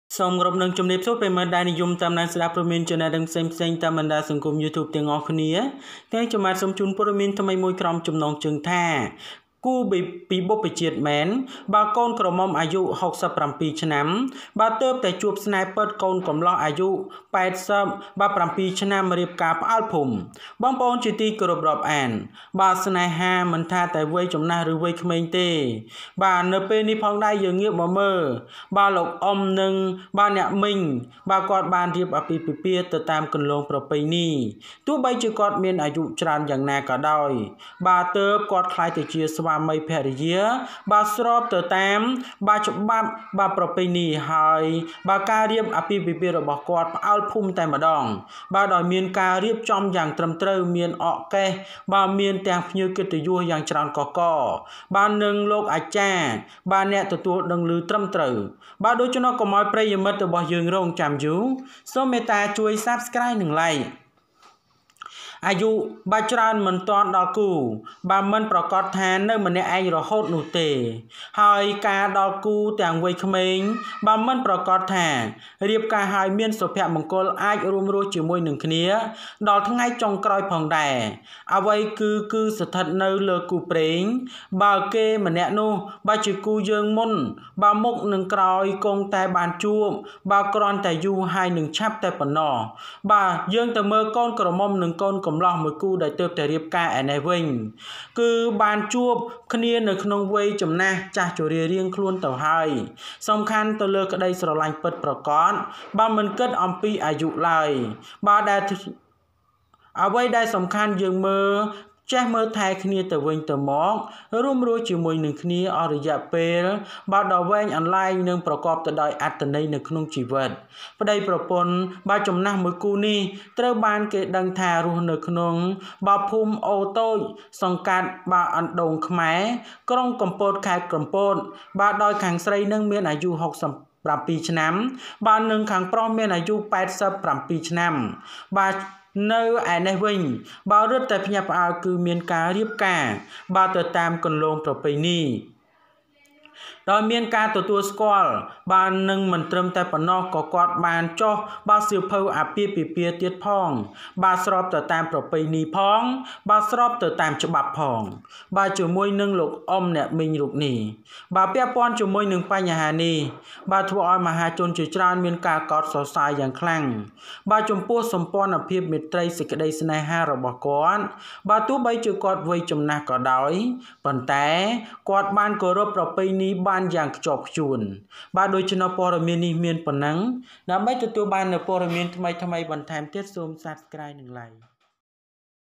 គូពីបុព្វជាតិ! កូនក្រមុំអាយុ ៦៧ ទើបជួបស្នេហ៍ពិតកូនកំលោះអាយុ ៨៧ រៀបការផ្អើលភូមិ
គូពីបុព្វជាតិ! កូនក្រមុំអាយុ ៦៧ ទើបជួបស្នេហ៍ពិតកូនកំលោះអាយុ ៨៧ រៀបការផ្អើលភូមិ Couple from the ancestors!  67-year-old bride meets true love, 87-year-old groom marries village.